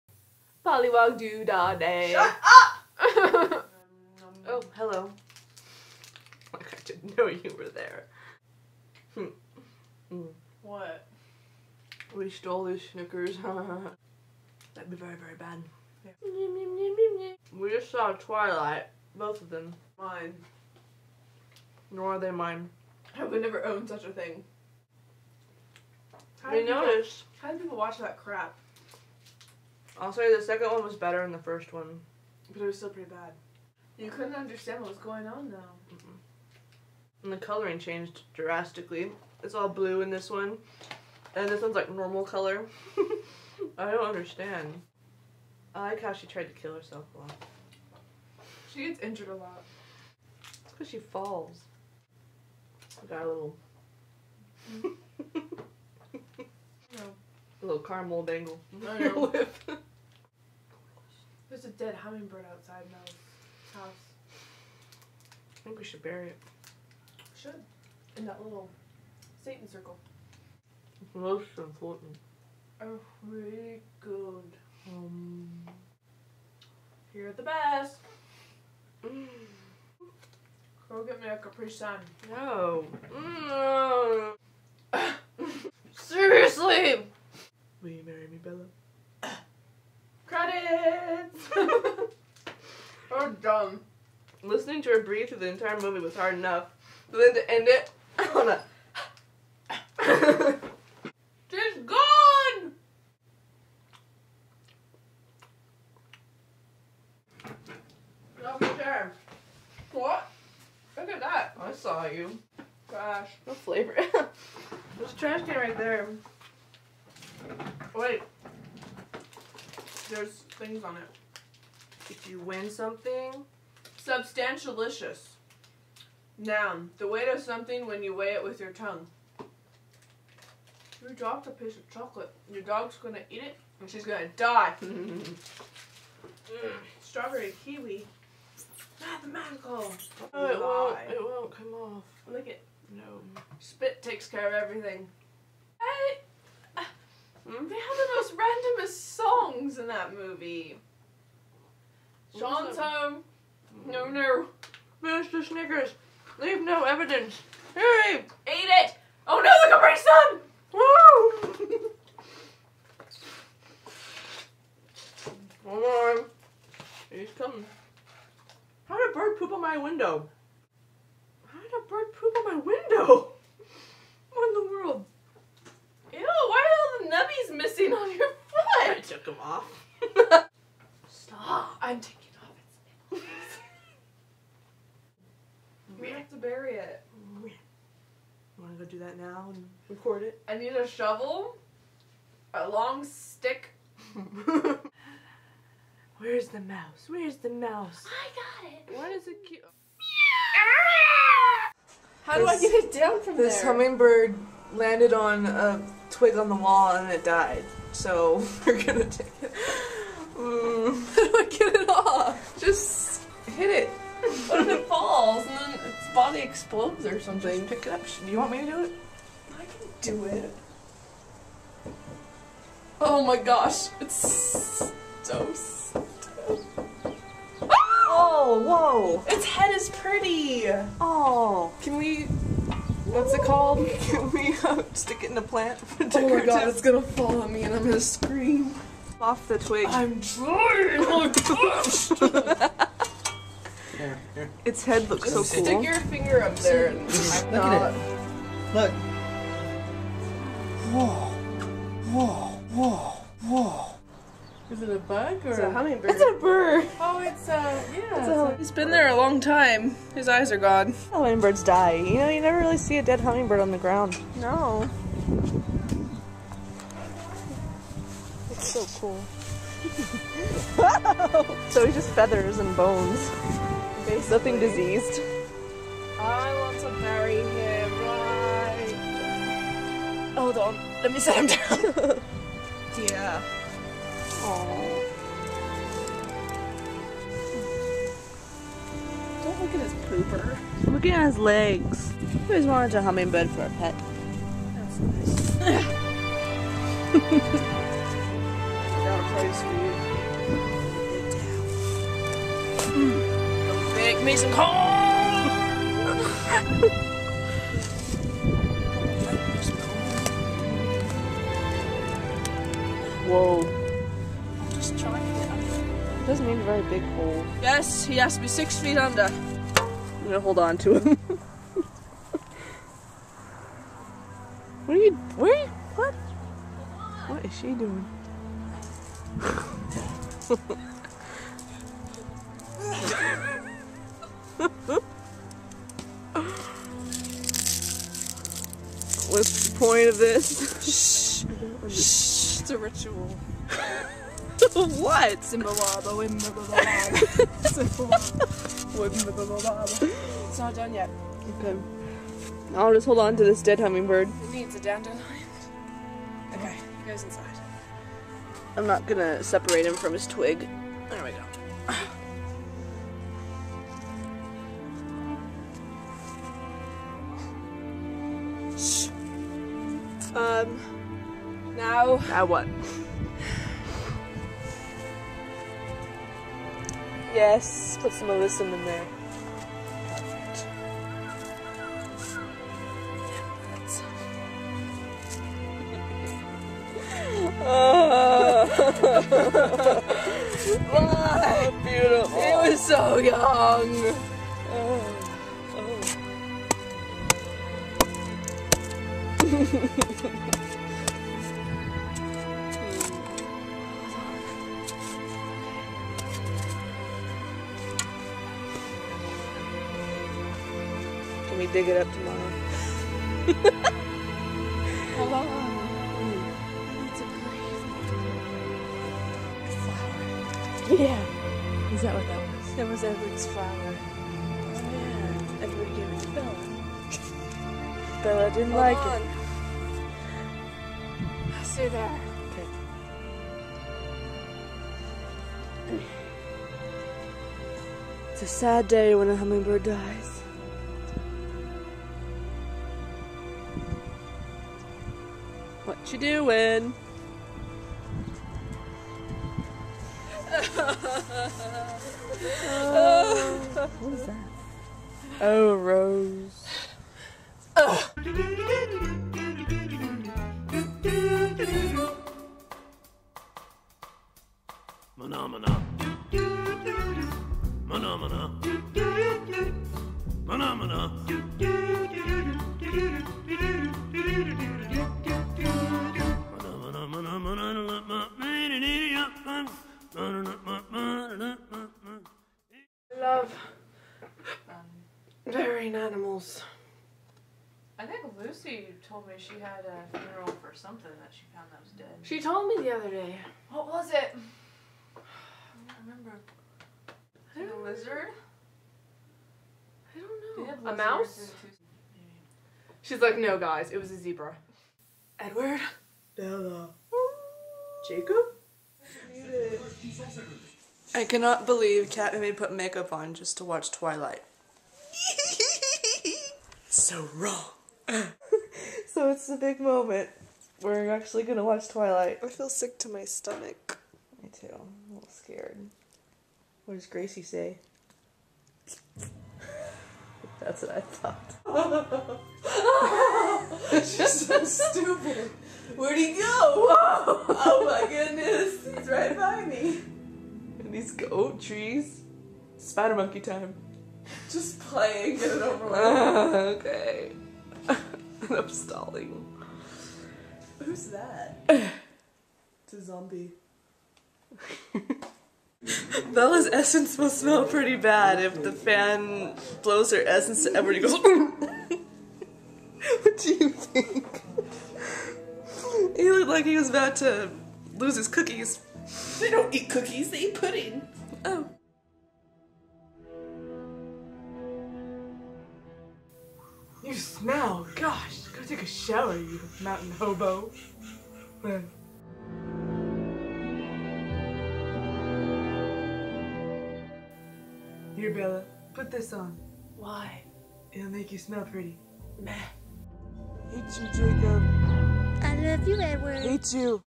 Pollywog doo da day
we watched Twilight and New Moon...its true we did. cause we know it sucks...but we still had to watch it, just to make sure. and found a dead humming bird attached to mel's house!

no credit was taken for the songs